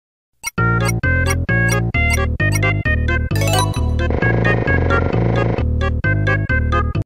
[Animation] Color it right | Pataware Entry
Entry for Patafoin's Collab, based on Wario Ware microgame. In this microgame, the player have to choose the right color to fill sonic missing's color